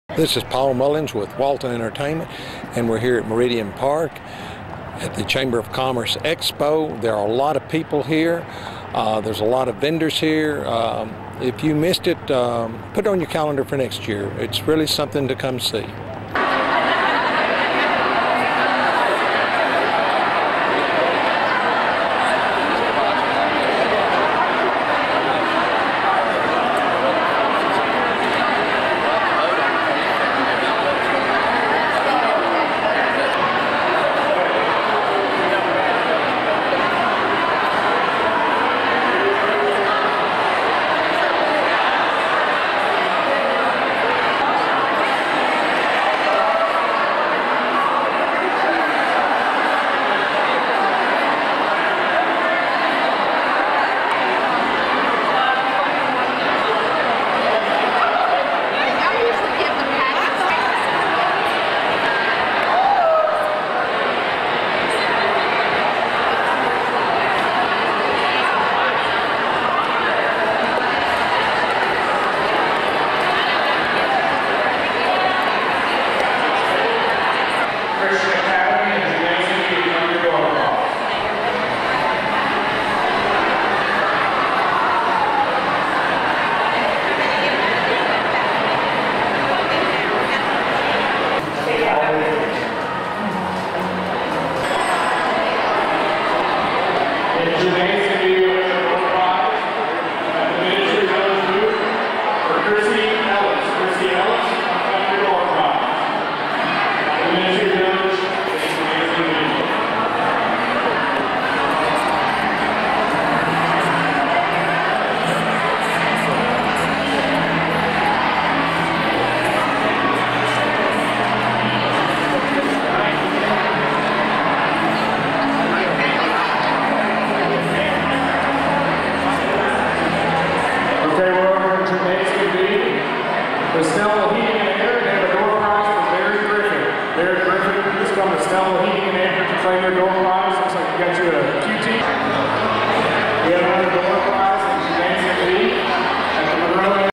Walton County Business Expo Oct 2012
This is an annual event put on by the Walton Chamber of Commerce. Spotlighting local businesses in your community.